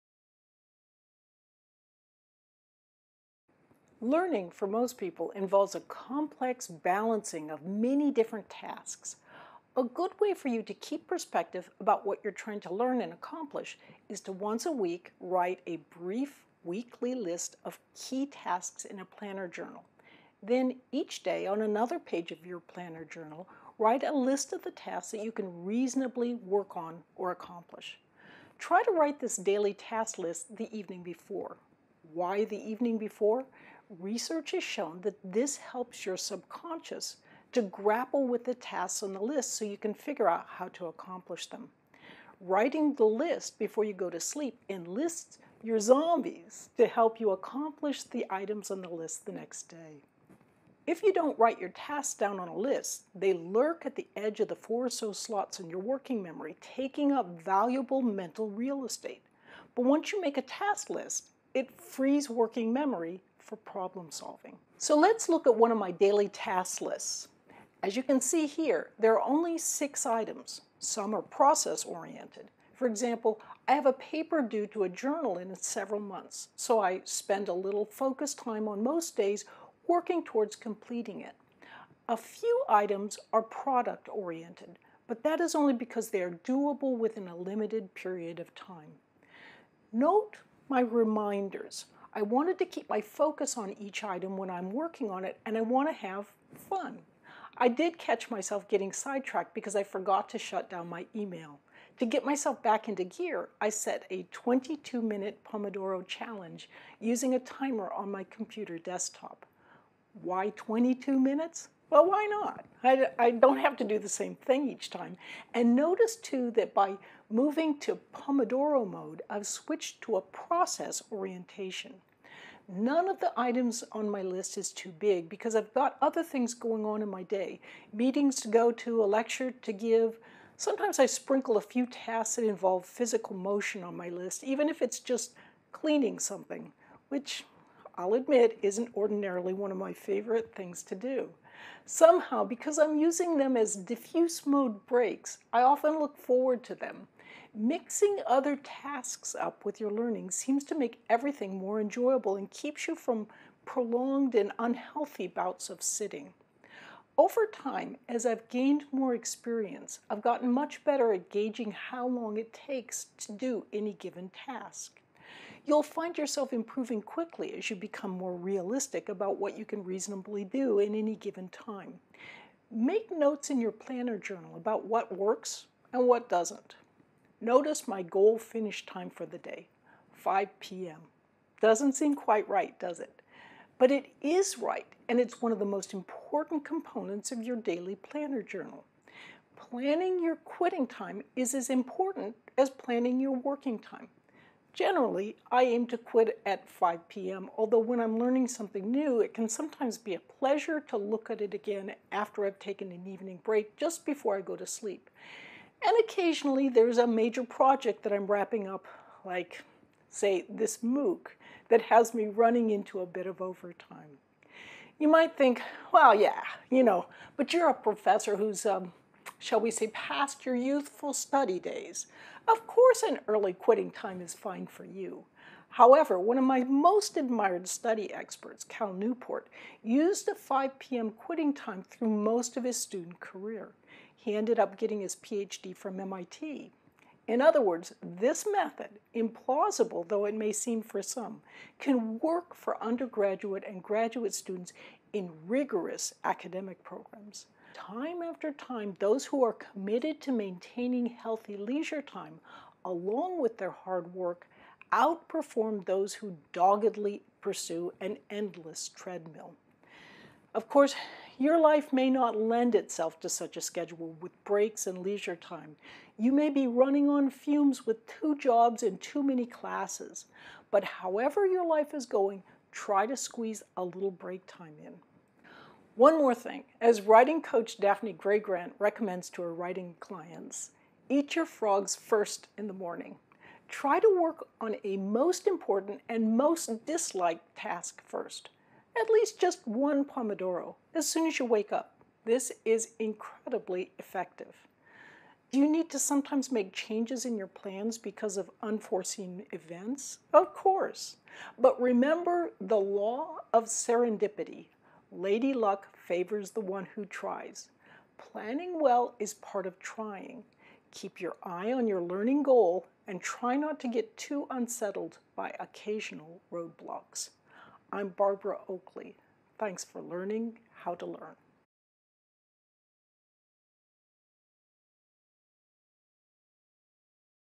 Juggling Life and Learning University of California, San Diego
In this module, we talk about two intimately connected ideas—procrastination and memory. Building solid chunks in long-term memory--chunks that are easily accessible by your short term memory—takes time. This is why learning to handle procrastination is so important. Finally, we talk about some of the best ways to access your brain’s most powerful long-term memory systems.